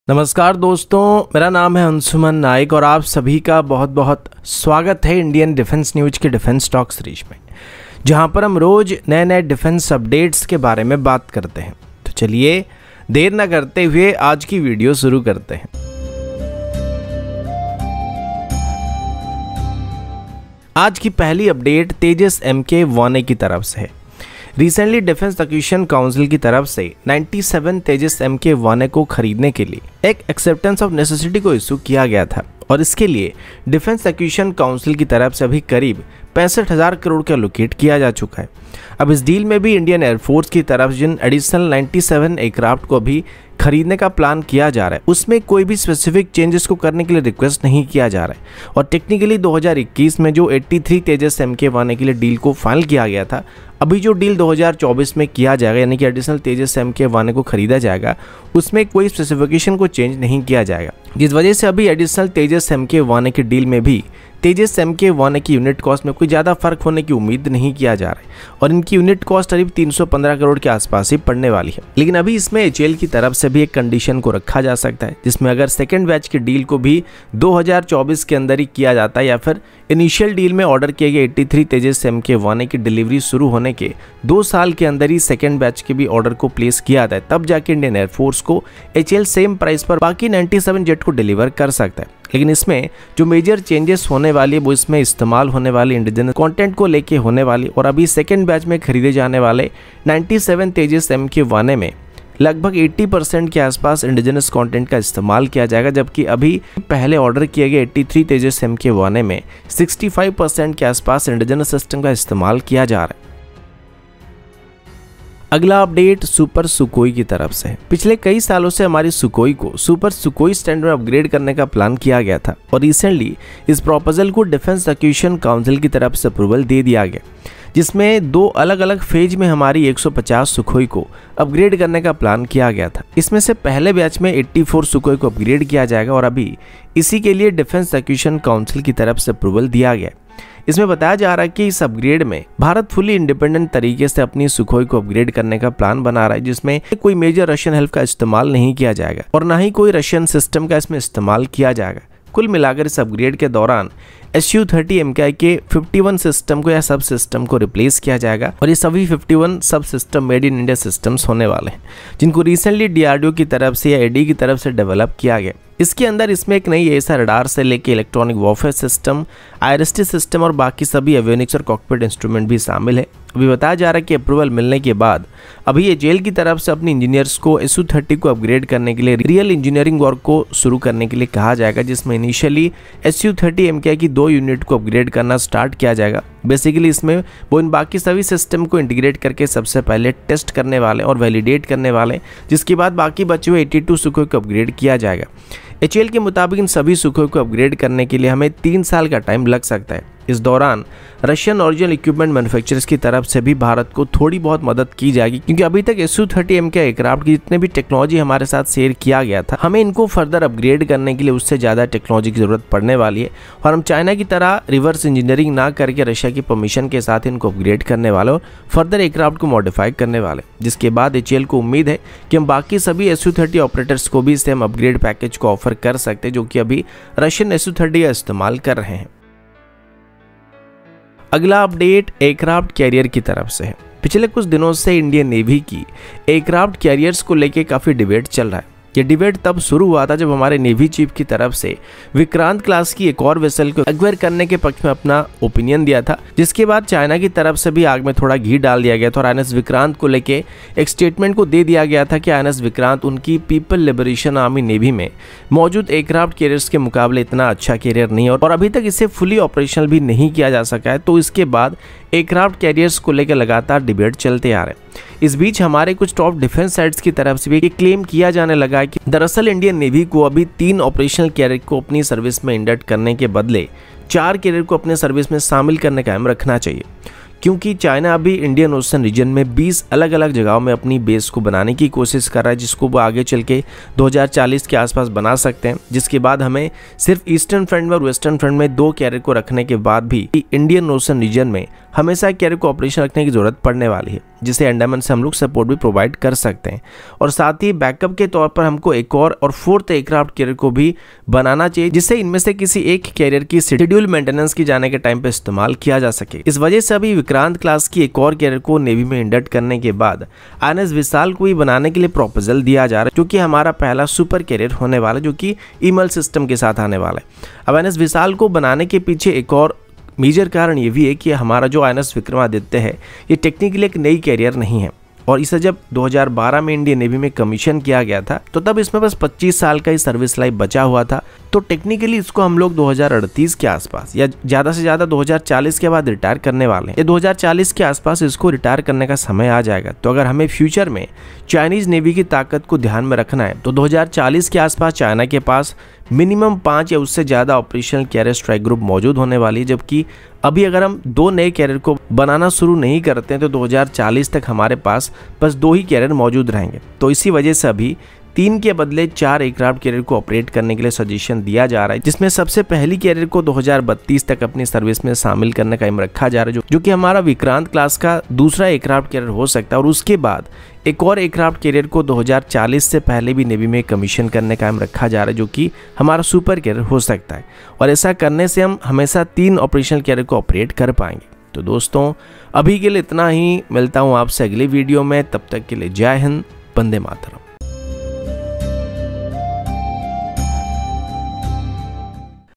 Indian Defence News:The Need of a 4th Aircraft Carrier,Super Su-30mki,Tejas Mk1a Unit Cost
Today's Latest Indian Defence News/Indian defence Updates in Hindi As Follow:Indian Defence News:The Need of a 4th Aircraft Carrier,Super Su-30mki,Tejas  Mk1a Unit Cost

0:00  -Intro / Start
0:20 - 97 LCA-Tejas Mk1A to cost same, Contract likely in 2024
1:53 - India to go solo on “Super 30” Upgrade Program ,Will breach Russian IPR
3:49 - India’s Defense Analyst Advocates for a Four-Aircraft Carrier Fleet